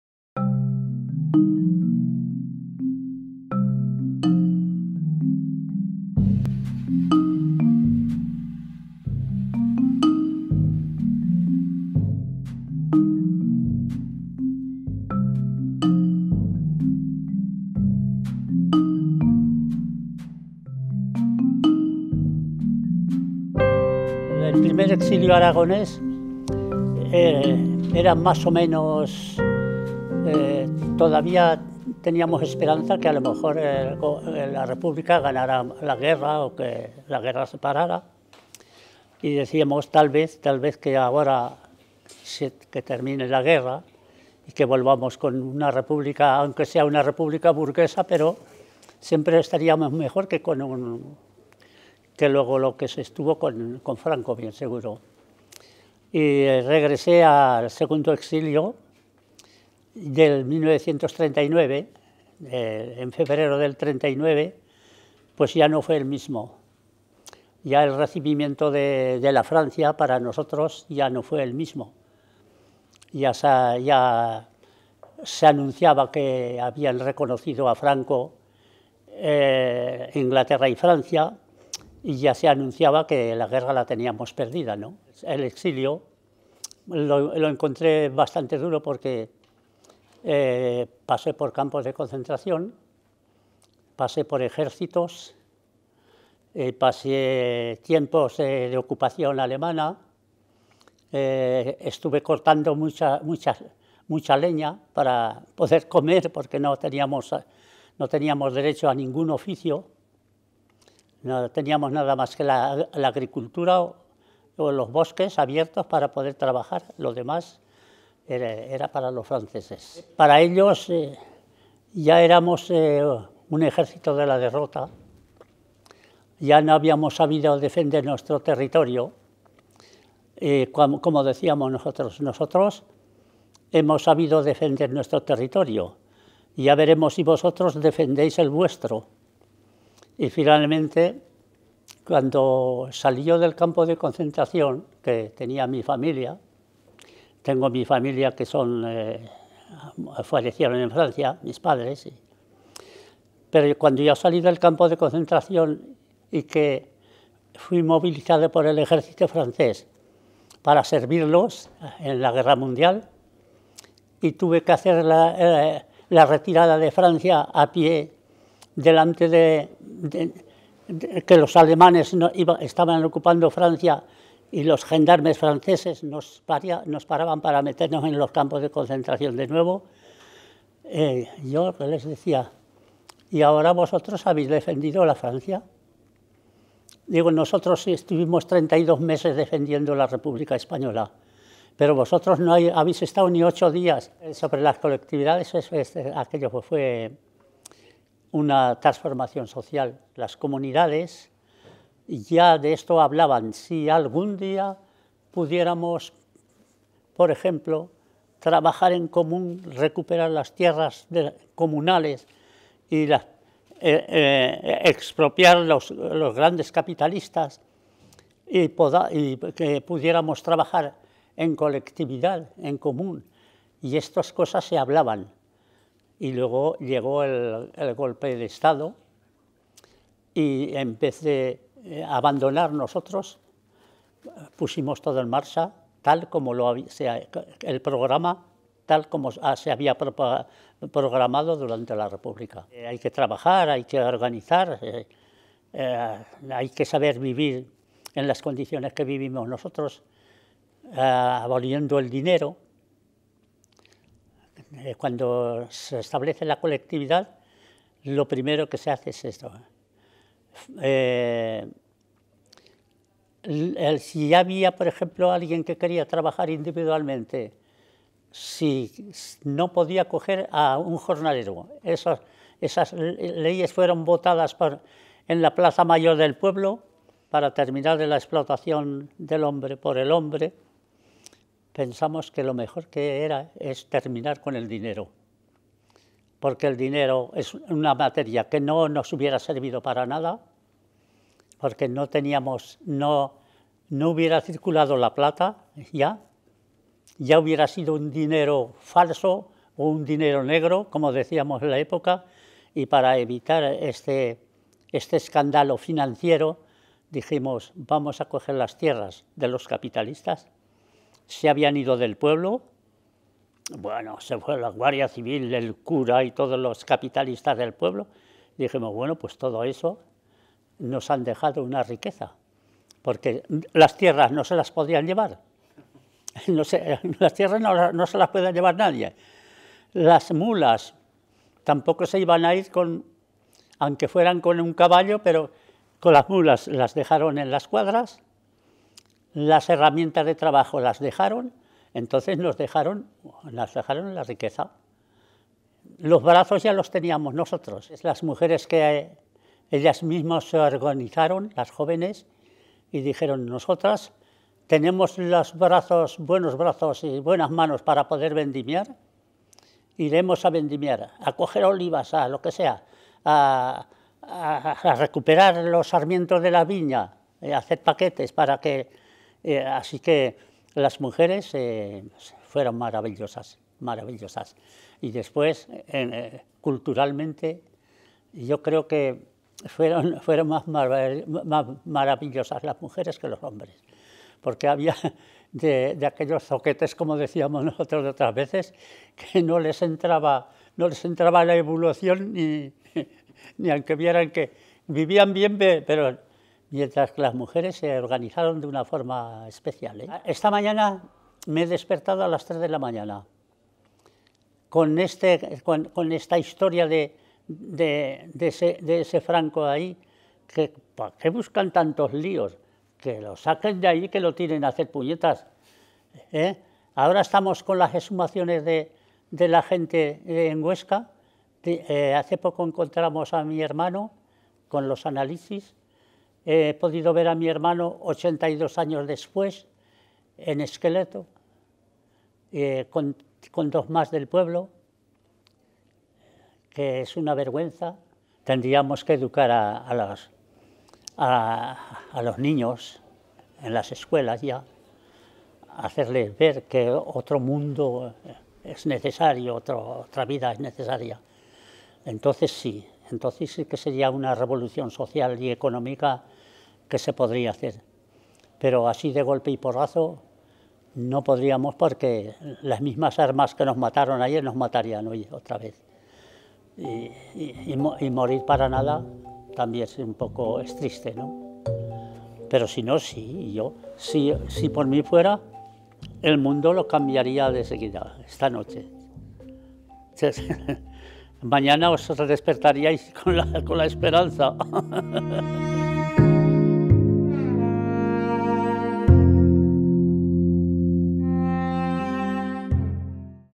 Entrevista a Martín Arnal
Tras vivir casi 40 años exiliado en Francia, Martín Arnal Mur volvió a España al morir Franco. Y se decepcionó. En su larga biografía hay espacio para la derrota, el dolor, las ilusiones perdidas y la esperanza. La llama del anarcosindicalismo, la experiencia de la colectividad agraria en su pueblo en 1936 y el paso por los campos de concentración franceses forjaron una vida que es todo un monumento a la dignidad y a la memoria.